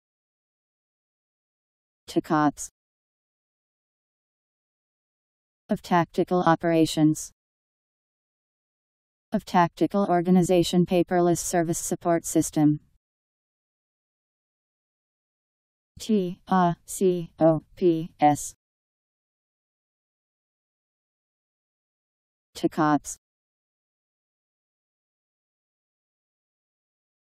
Tacops Meaning
Video shows what tacops means. of tactical operations. of "Tactical Organization Paperless Service Support System"..  Tacops Meaning. How to pronounce, definition audio dictionary. How to say tacops. Powered by MaryTTS, Wiktionary